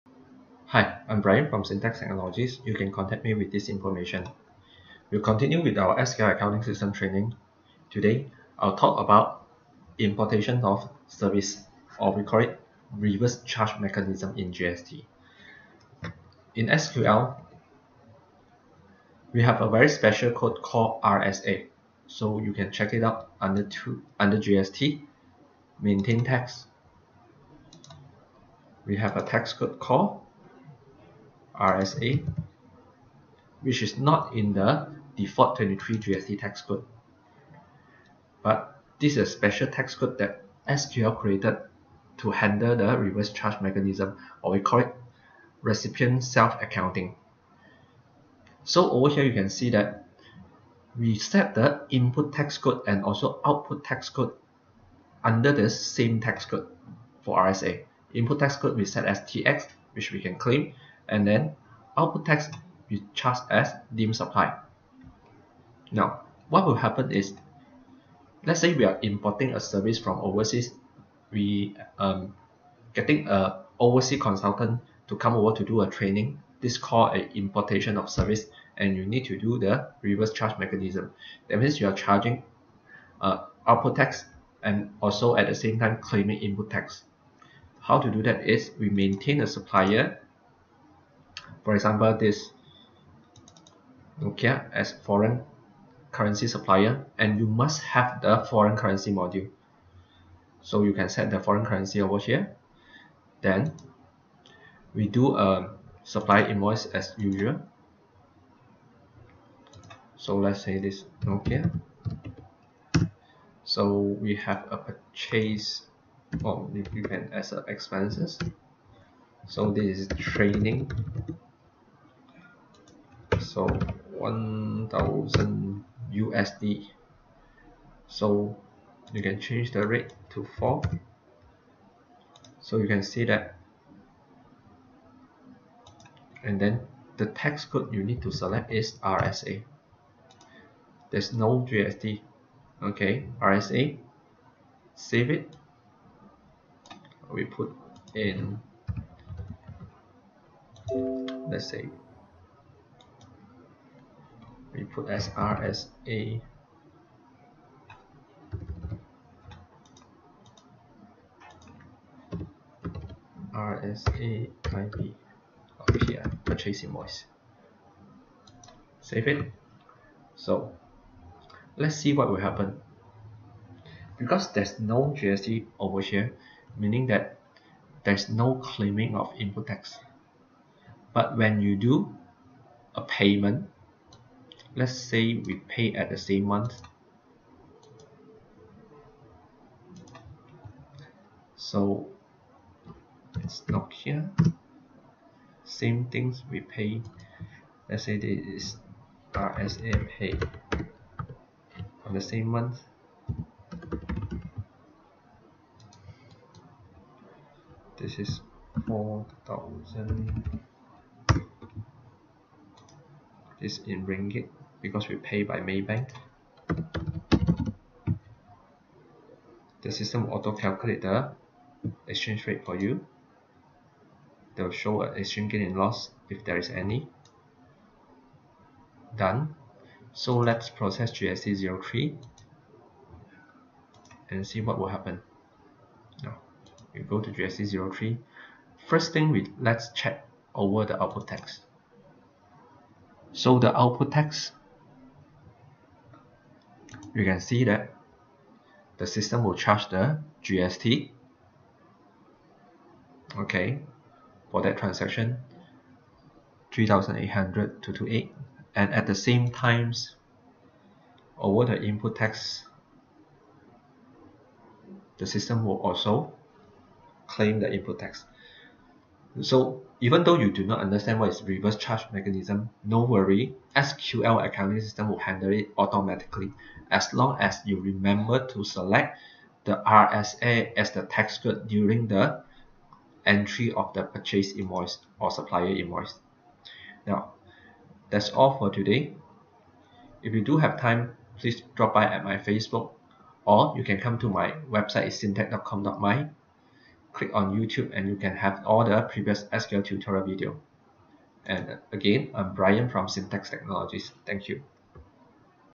SQL ACCOUNTING SYSTEM GST 063 IMPORTATION OF SERVICE OR REVERSE CHARGE MECHANISM
Reverse Charge Mechanism is one of the difficult concept to be understand in GST. I'll show you via SQL Accounting System, how this concept work.